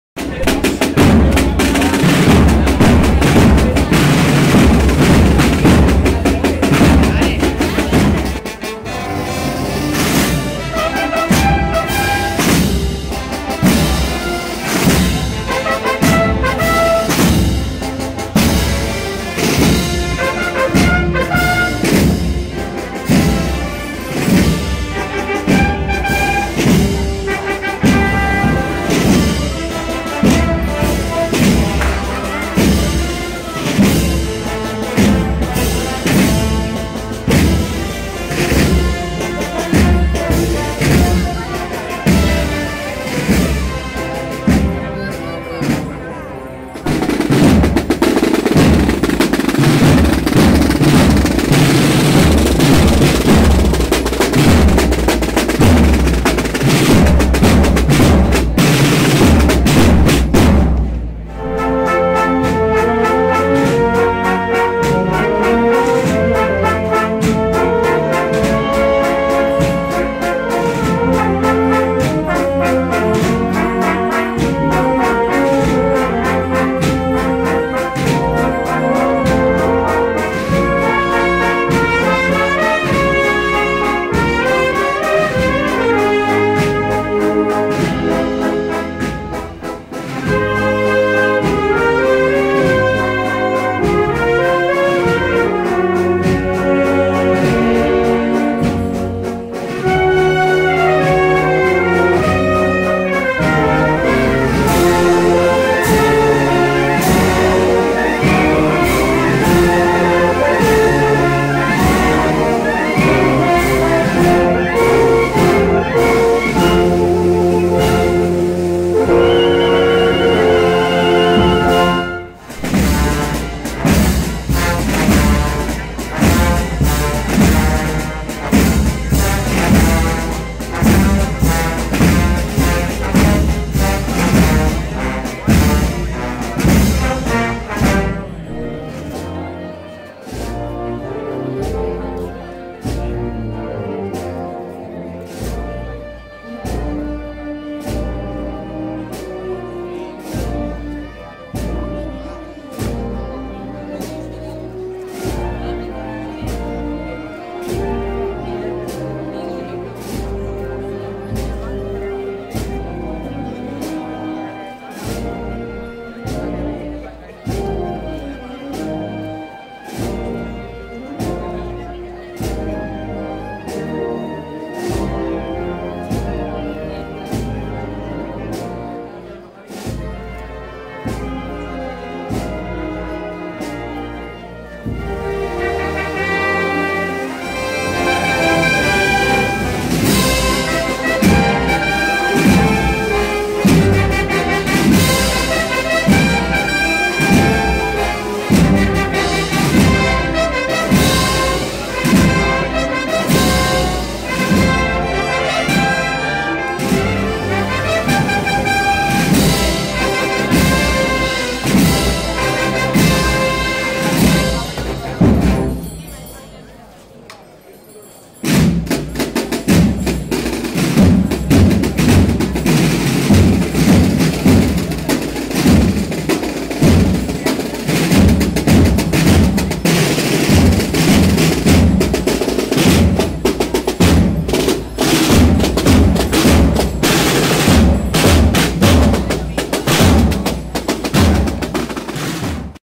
Virgen de Aguas Santas | BM Las Cigarreras | 2019
Procesión de Nuestra Señora de Aguas Santas de Villaverde del Río la noche del 8 de septiembre.

Banda de Música María Santísima de la Victoria